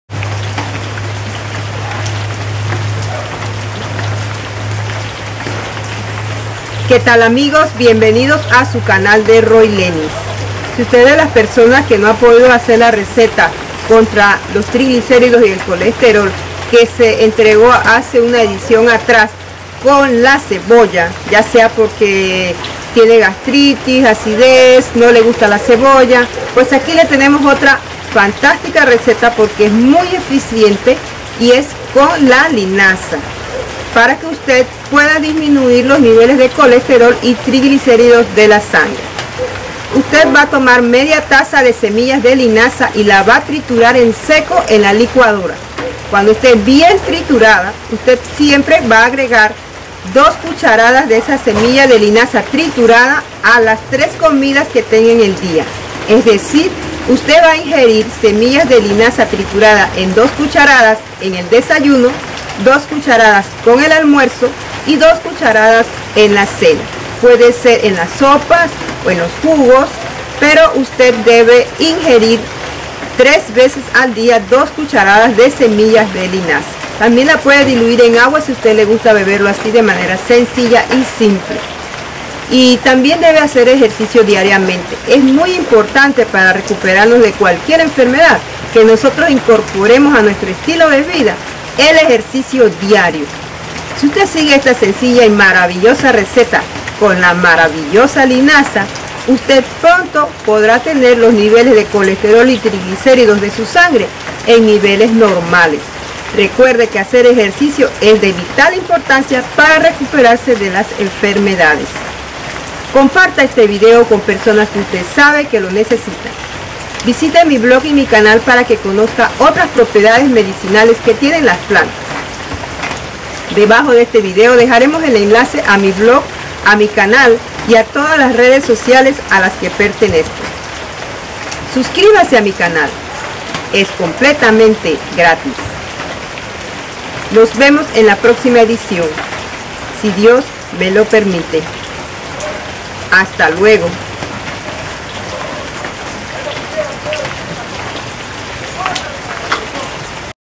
Cómo Bajar Eficazmente El Colesterol Y Los Triglicéridos
La linaza, es altamente efectiva para bajar los niveles de colesterol y triglicéridos de la sangre.  En esta receta, encontrarás una manera efectiva y sencilla para ingerirla en las dosis apropiadas para bajar los niveles de colesterol y triglicéridos de la sangre a niveles normales.
ES DE VITAL IMPORTANCIA ABANDONAR LOS ALIMENTOS DE ORIGEN ANIMAL Y EL AZÚCAR DE SU DIETA, DE LO CONTRARIO NO SE RECUPERARÁ DE SU ENFERMEDAD.